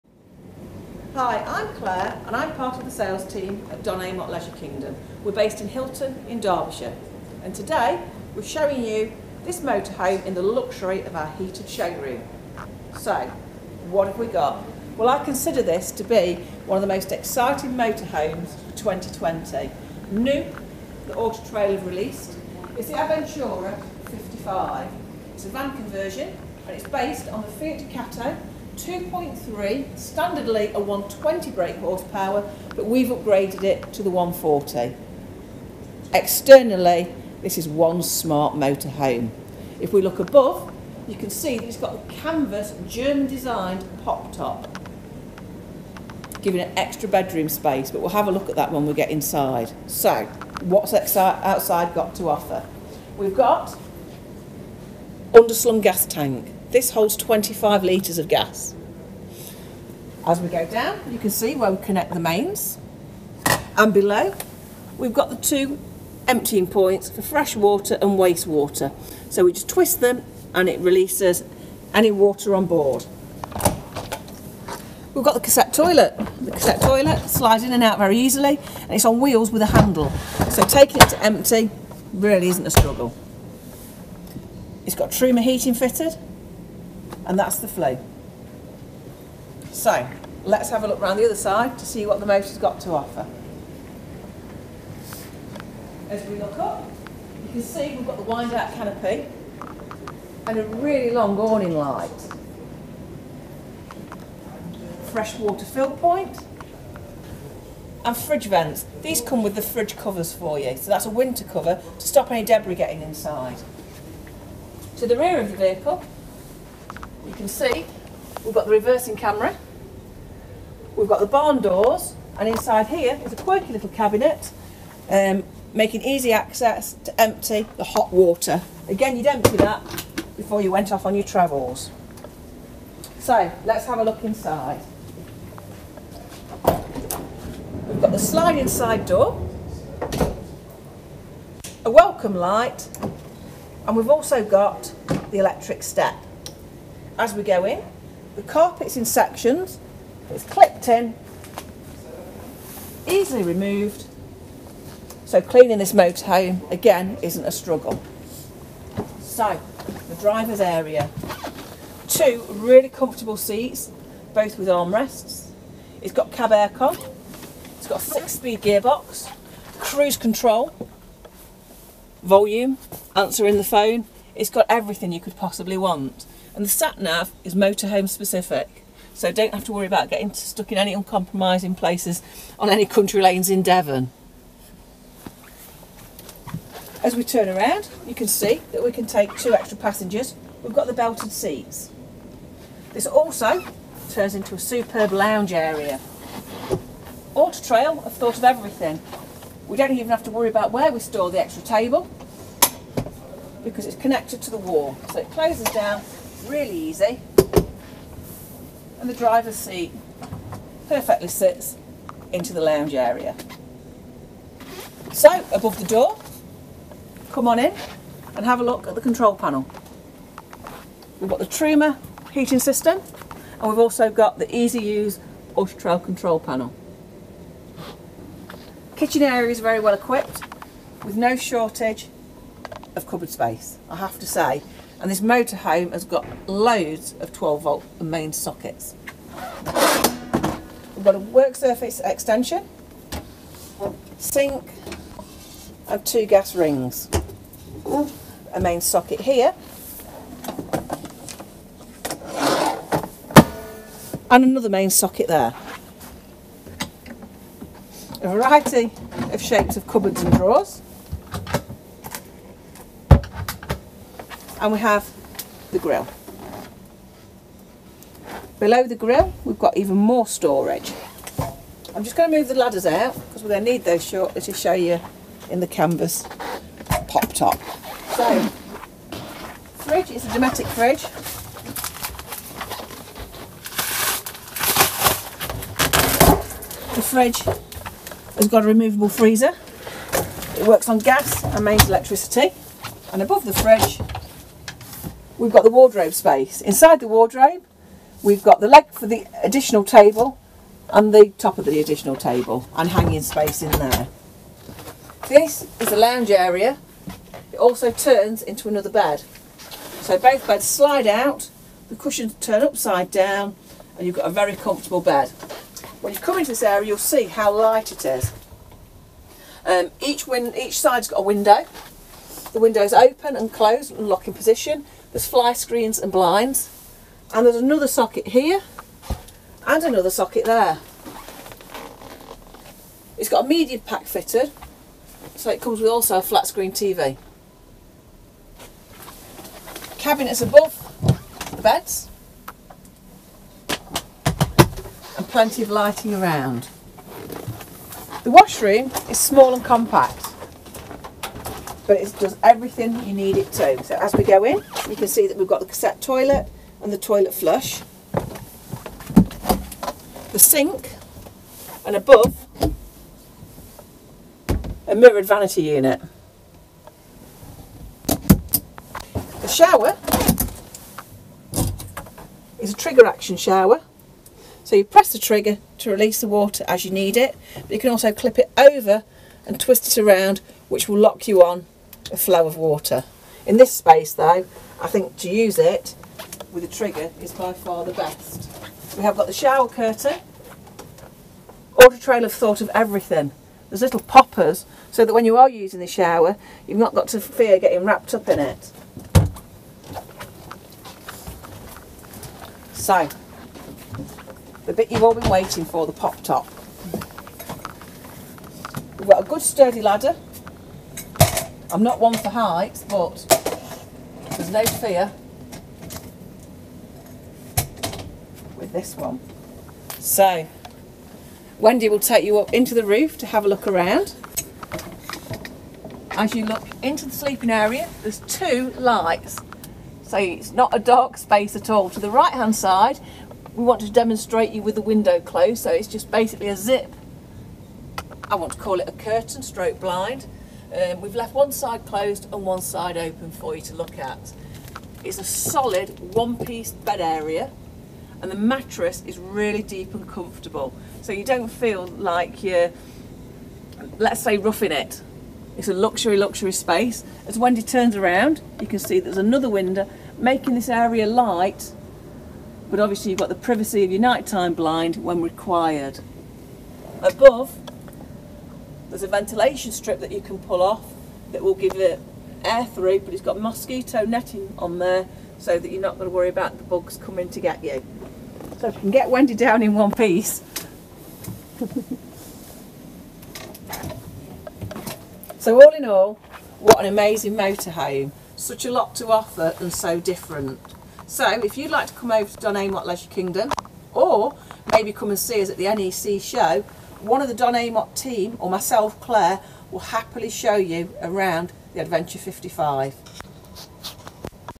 Auto-Trail Adventure 55
** BRAND NEW 2020 MODEL**
 This Auto-Trail Adventure 55, is built on the Fiat Ducato 2.3 litre 140bhp engine with a 6-speed manual transmission and in the 'Grigio Grey'.
This newly developed range ensures freedom to roam on any voyage. Suitable for a family of four, the Auto-Trail Adventure is equipped with the most purposeful of features both inside and out. You ll be hitting the road on 16 diamond cut alloy wheels and black coded front grille.
The Auto-Trail Adventure leisure vehicles come with the Media Pack as standard, meaning you ll be relaxing in comfort with a 19.5 Avtex wide screen TV/DVD incorporating digital and satellite tuners, a new Zenec LCD head unit featuring a motorhome specific SAT NAV as standard, digital radio with integrated microphone, colour reversing camera, plus steering wheel radio controls and a status TV aerial with digital signal finder. Never pass the opportunity to take the family on a new adventure, as each layout presents travelling seats for four, a gas strut assisted elevating insulated roof, incorporating a luxurious double bed with comfort springing, ventilation, reading lights and USB charge points.
On the ground floor there is a spacious rear lounge, which conveniently converts into another double bed come night time. A solar panel is fitted as standard, meaning those spontaneous wild camping escapades will be no problem for the Auto-Trail Adventure. These pop top campervans are perfect for those who want to keep their leisure vehicle easily on the drive, with the ability to extend the roof when out and about. You can travel to your hearts content and be rest assured you ll experience a comfortable touring journey, no matter your destination.
Why not come to Don Amott Leisure for a full demonstration from our expert Sales Team on this Auto-Trail Adventure 55.